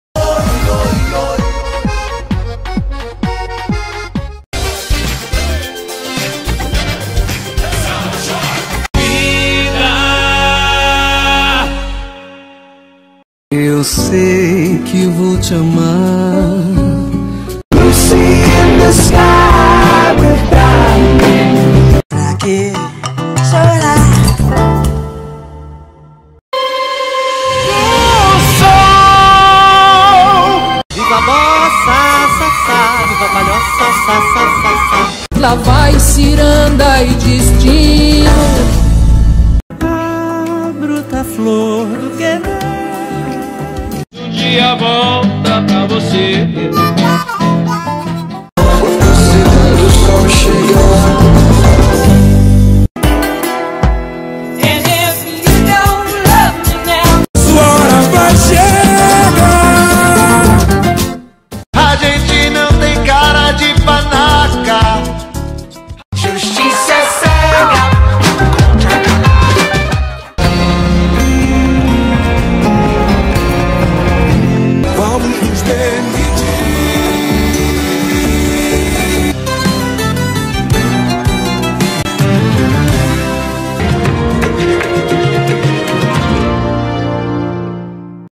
Novelas das 9
De Avenida Brasil á Renascer, qual a sua preferida e a menos boa na sua opinião?